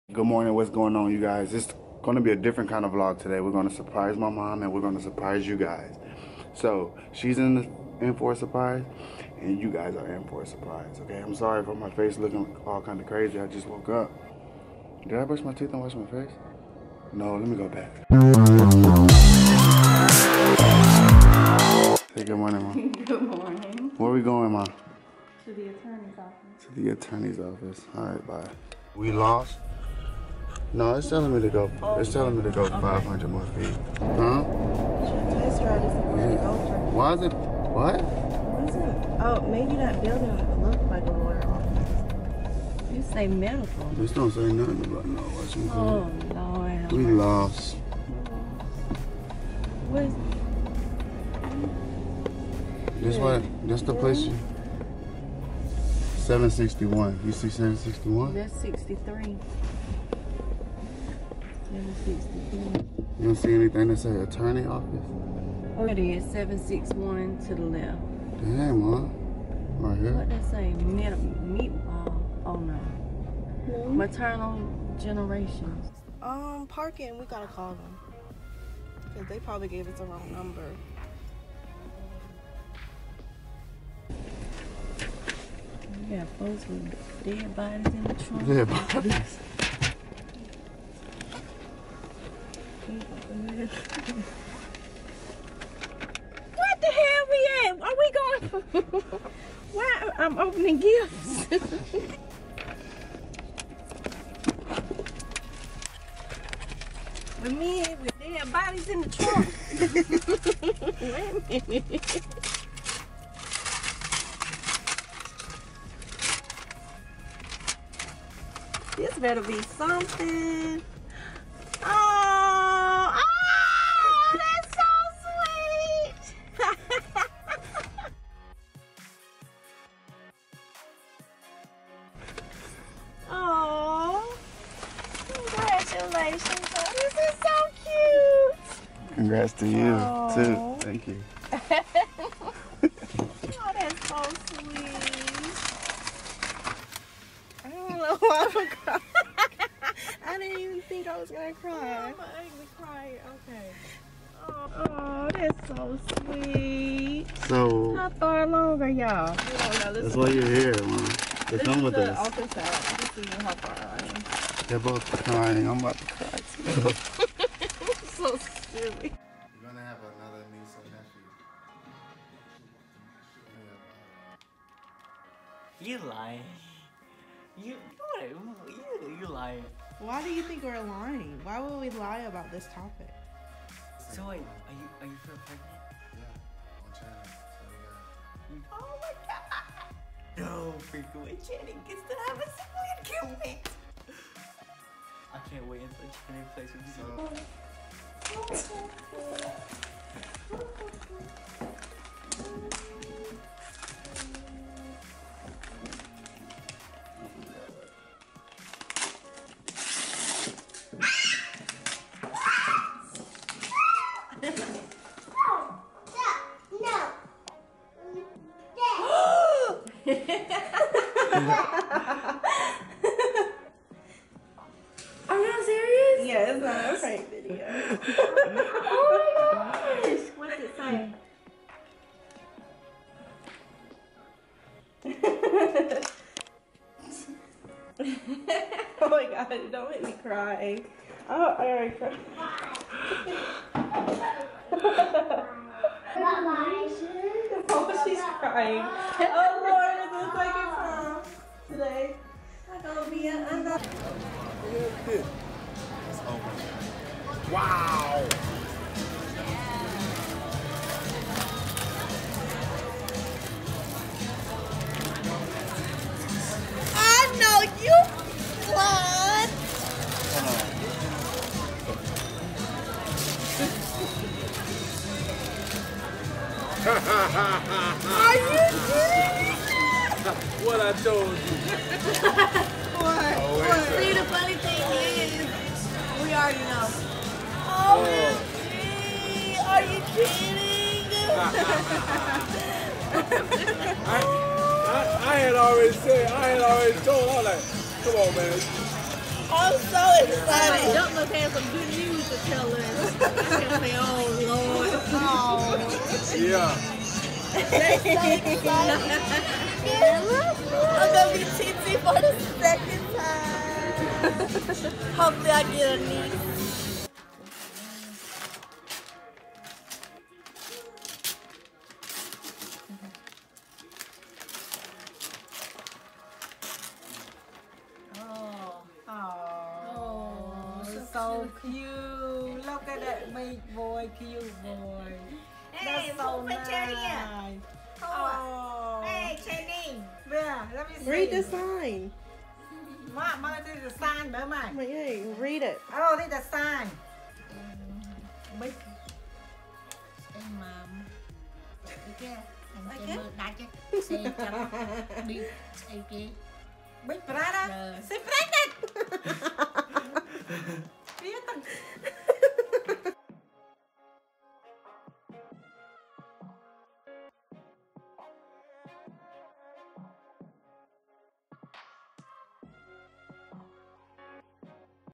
SURPRISE PRANK ON MY MOM!! (& MY WHOLE FAMILY!)
Watch me prank my mom and the rest of the family with the BIGGEST news! I told her we were meeting with the attorney's to get her to come!